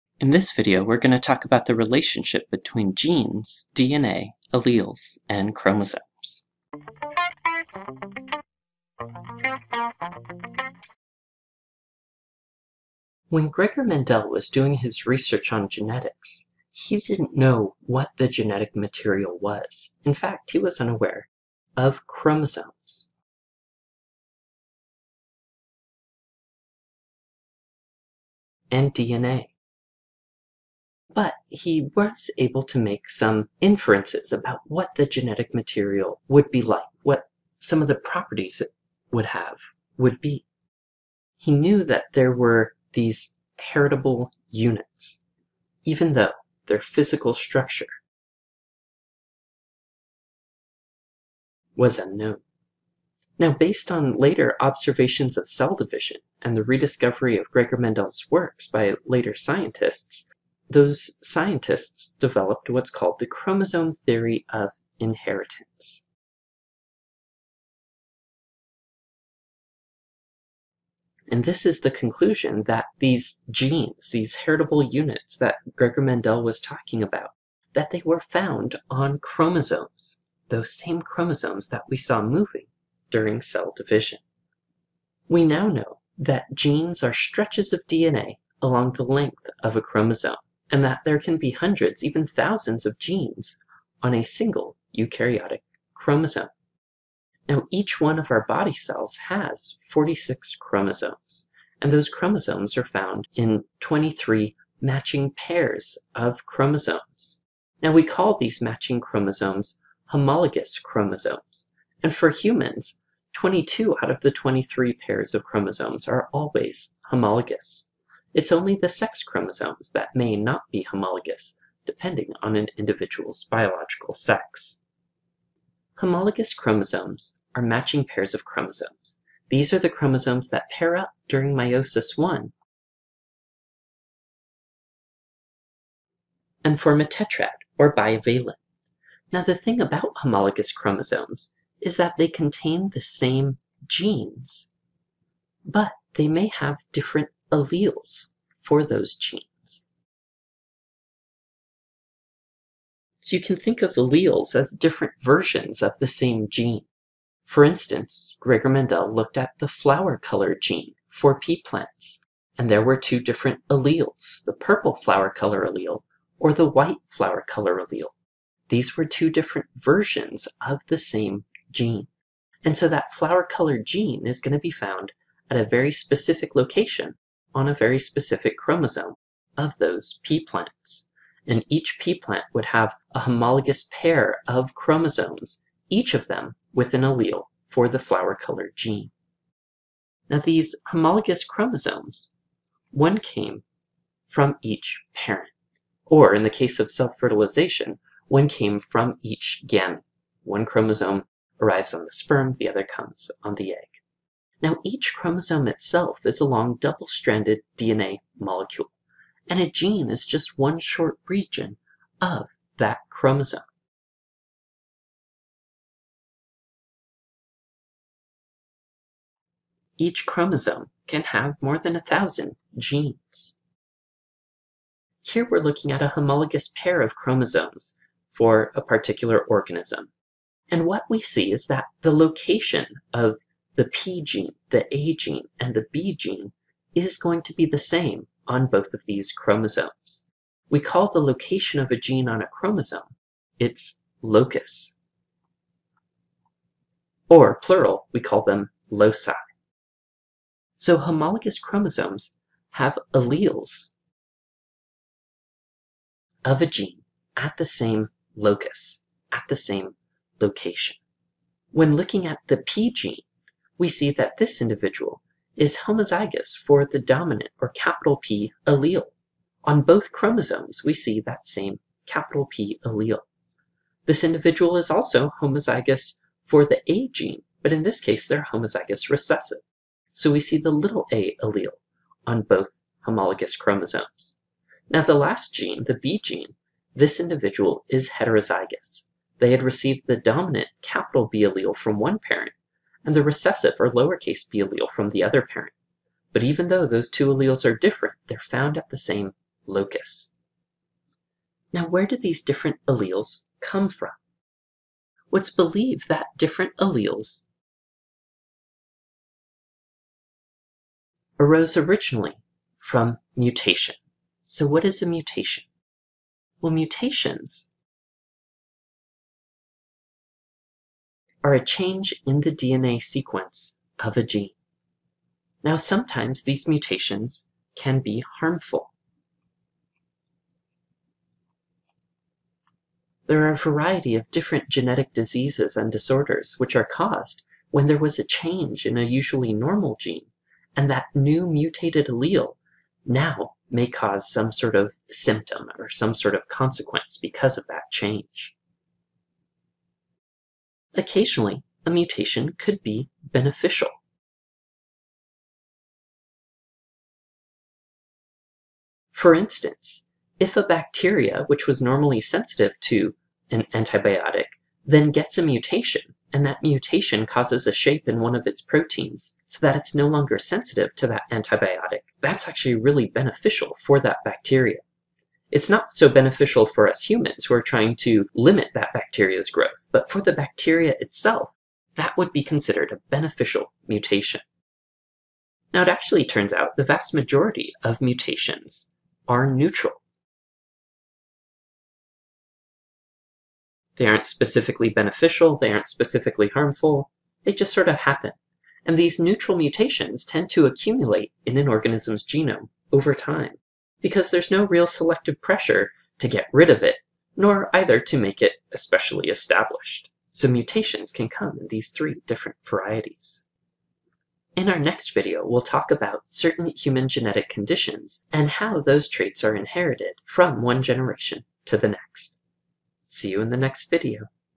DNA, Chromosomes, Genes and Alleles – 8.1 – Biol 189
The purpose of this video is to introduce the viewer to the relationship between DNA, chromosomes, genes, and alleles. Blank Lecture Notes available in three formats